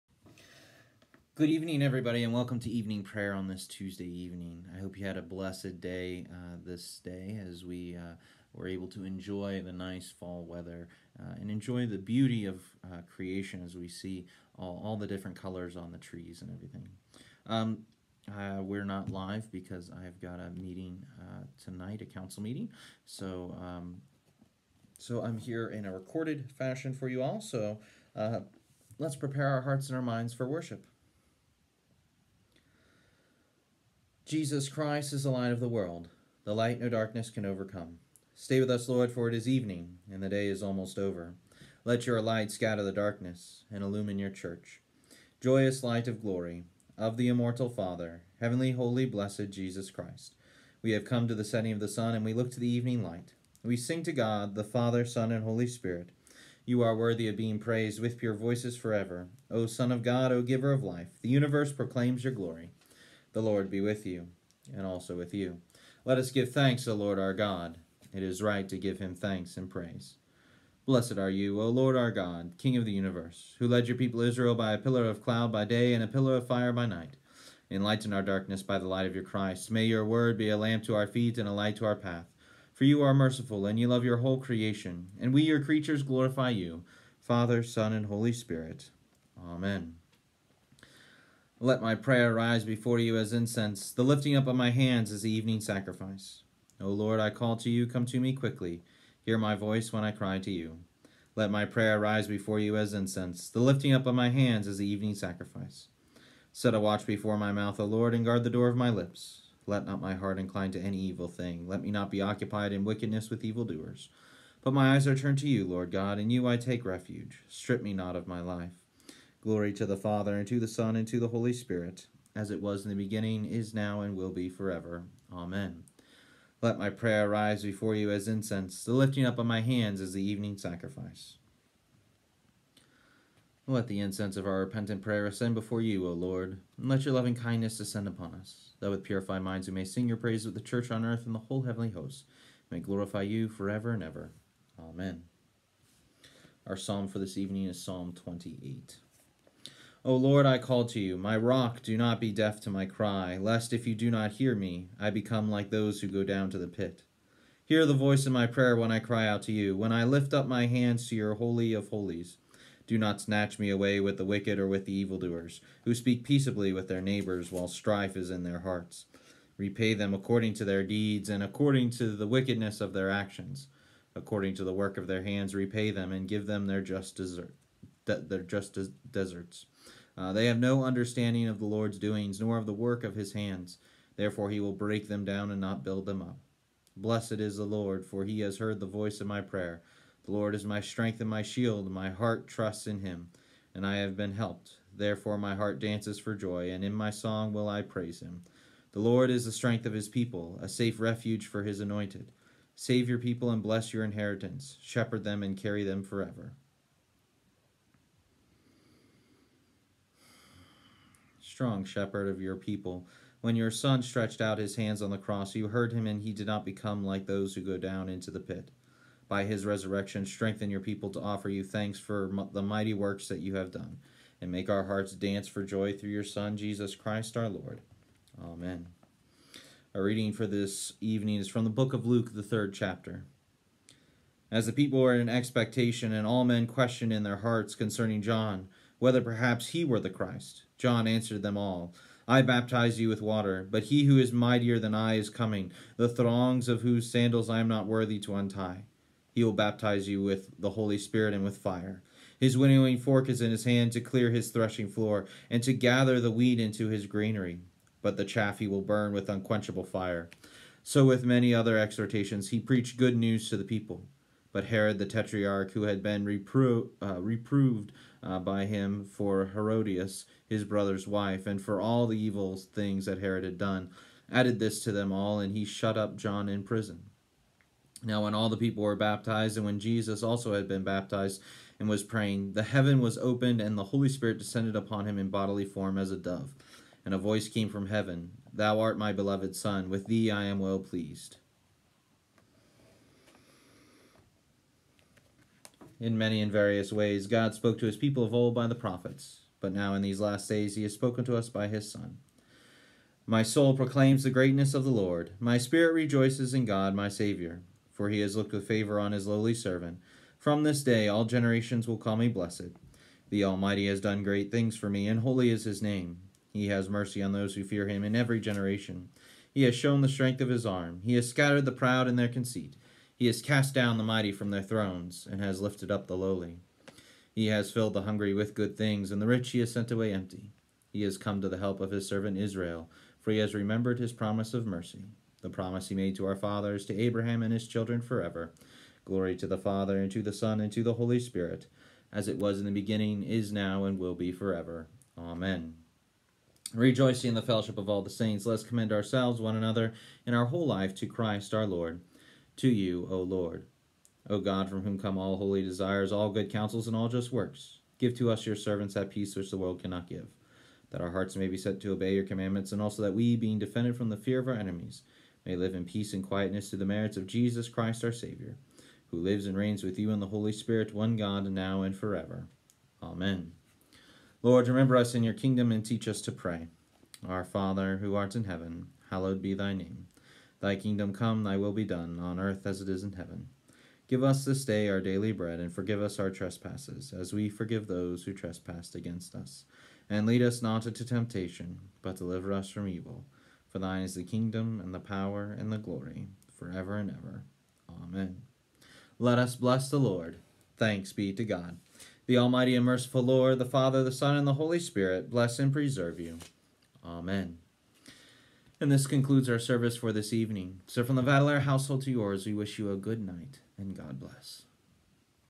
Evening Prayer 10/13/2020
Join in with Pastor Matthew as he leads us in Evening Prayer.